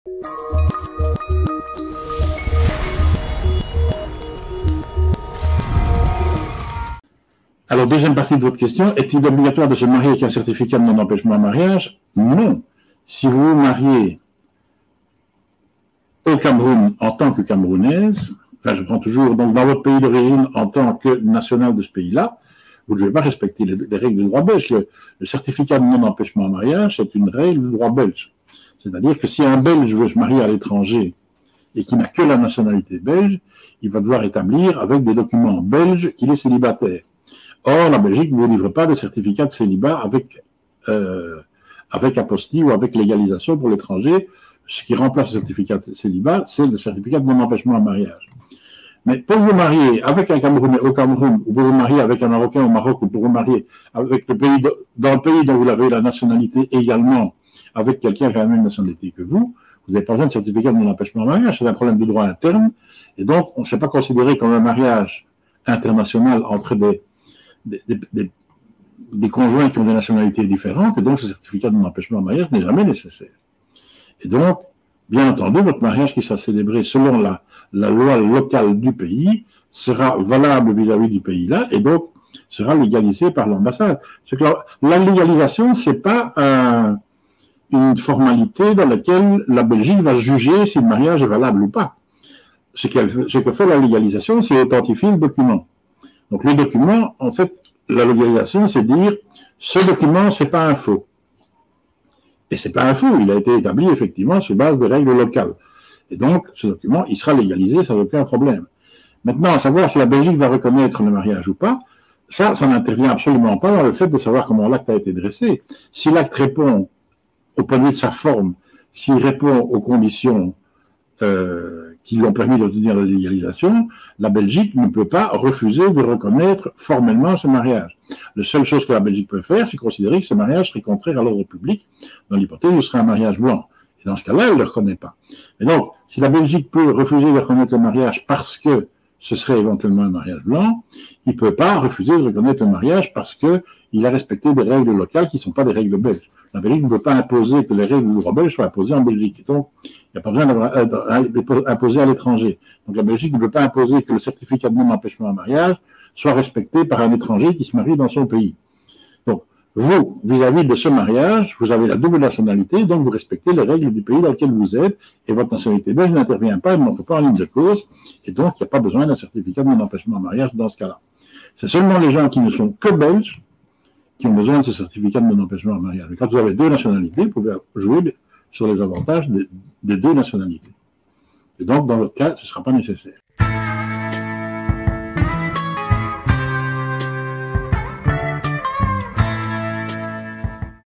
Un certificat de non-empêchement indispensable pour un belge qui désire se marier à l’étranger ?
Extrait de l'émission quotidienne "Demande plutôt à Michel" dans laquelle je réponds en direct à toutes vos questions en droit des étrangers.